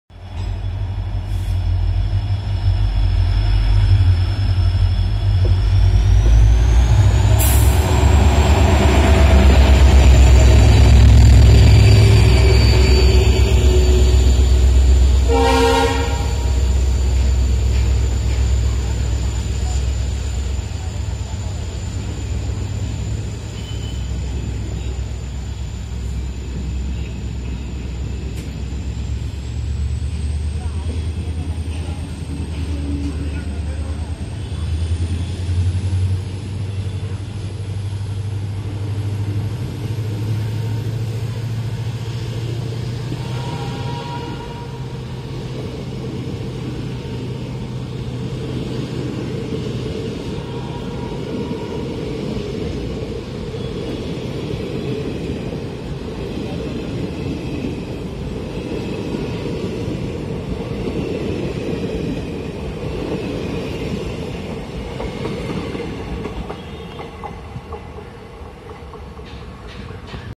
06274 - Arsikere - KSR Bengaluru Passenger Special was spotted near Yesvantpur Junction
06274 - Arsikere - KSR Bengaluru Passenger Special was spotted near Yesvantpur Junction, Bengaluru on July 23rd 2022

This train was hauled by Krishnarajapuram (KJM) WDP-4D #40551

Enjoy EMD humming and horn